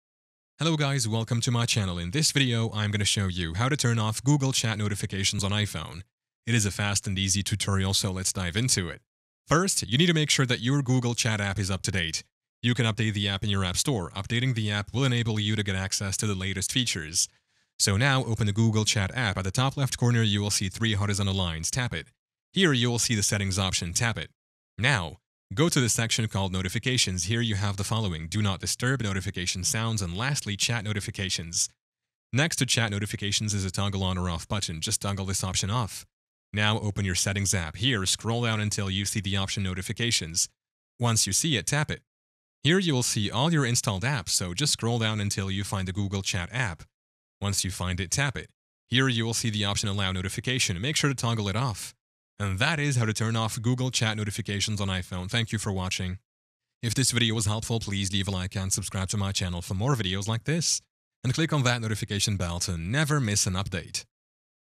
How To Turn Off Google Chat Notifications On iPhone (2025) - Easy Fix
In this video, we will explain to you about: How To Turn Off Google Chat Notifications On iPhone (2025) - Easy Fix